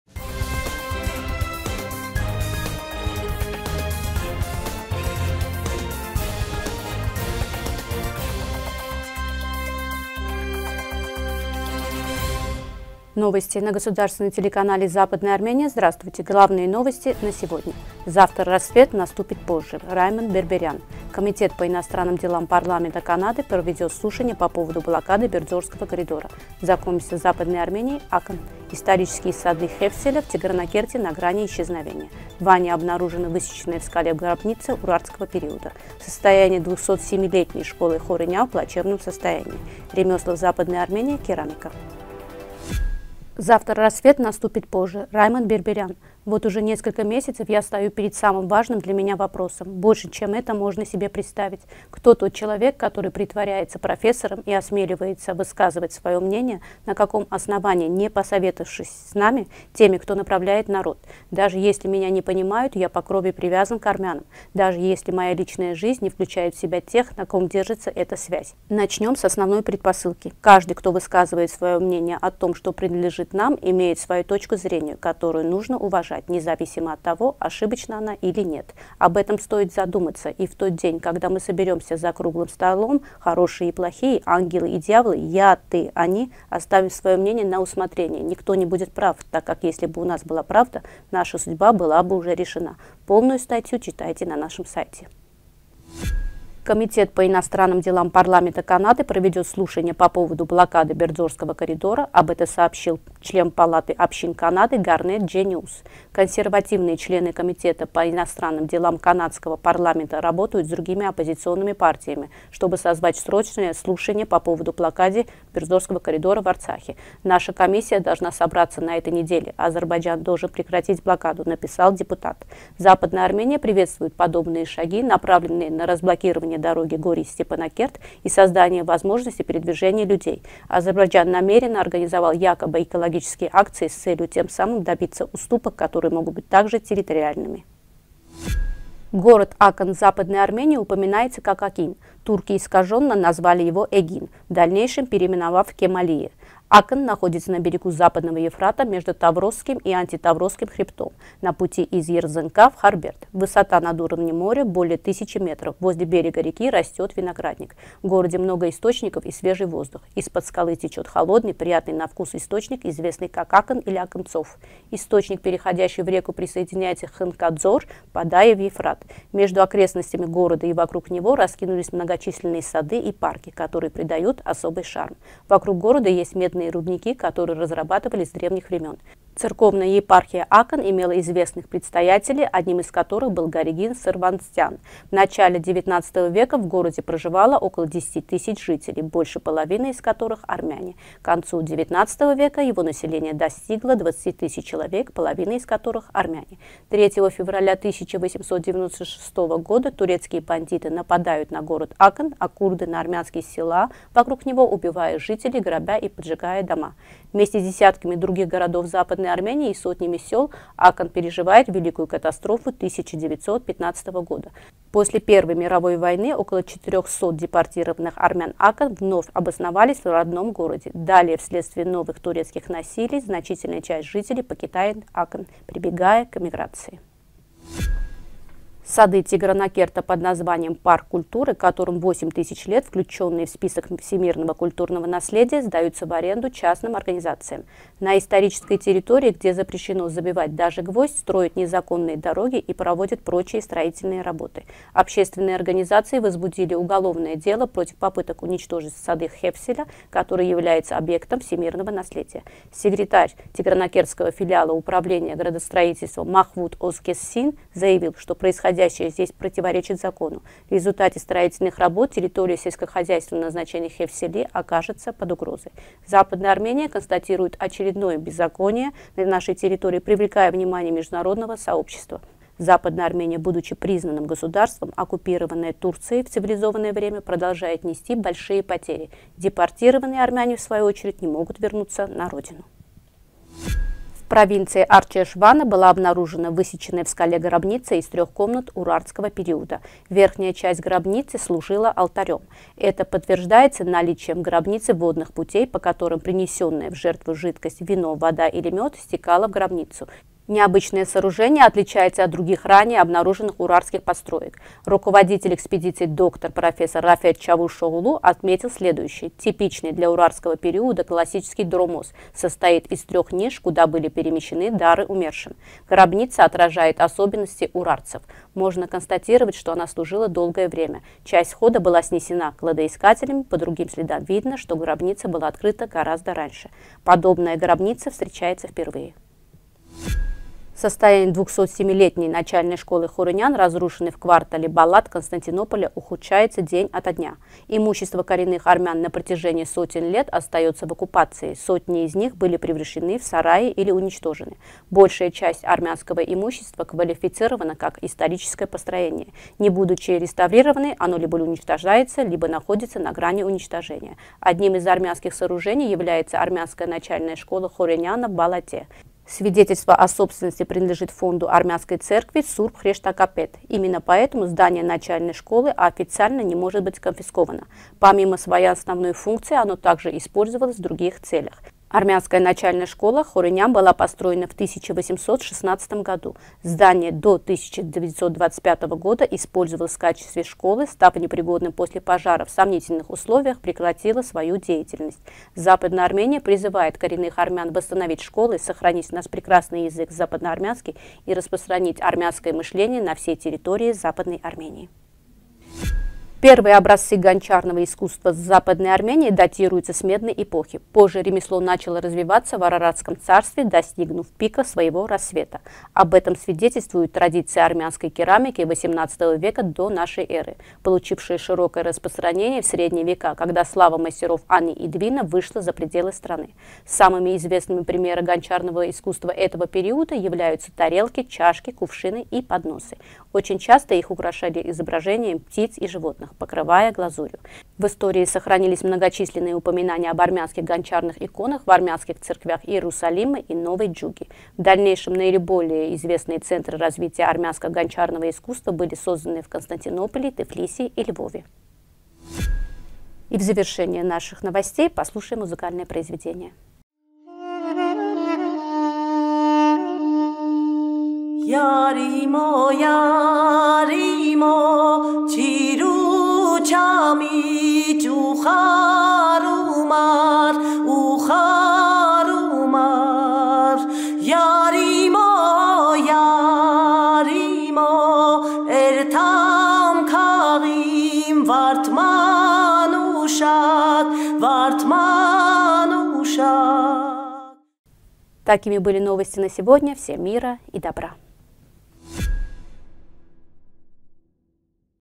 Завтра рассвет наступит позже
1. Завтра рассвет наступит позже
2. Комитет по иностранным делам Парламента Канады проведет слушания по поводу блокады Бердзорского коридора.
3. Знакомимся с Западной Арменией . Акн
4. Исторические сады Хевселя Тигранакерта на грани исчезновения
5. В Ване обнаружена гробница Урартского периода, высеченная в скале
6. Состояние 207 - летней школы Хоренян в плачевном состоянии
7. Ремесла в Западной Армении. Керамика